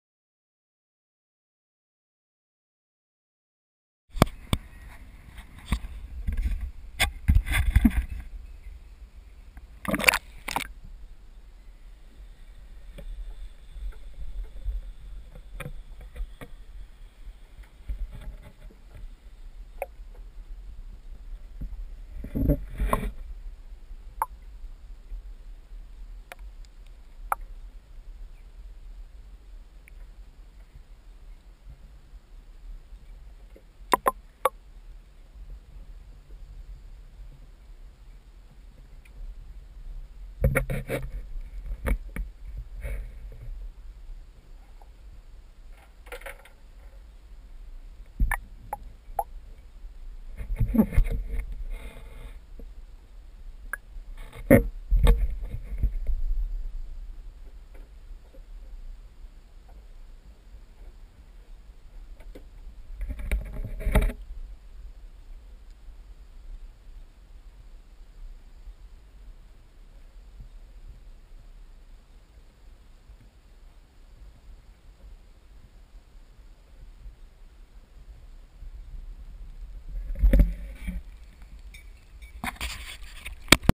GoPro HD Hero DIY Focus Fix
I laser cut acrylic to make a spacer, and a flat lens, then gave the housing a surface cut on a vertical milling machine to remove the o-ring retaining ridge. Then glued them all together.

This is the first time i tested it underwater, and it was shot in R5 mode.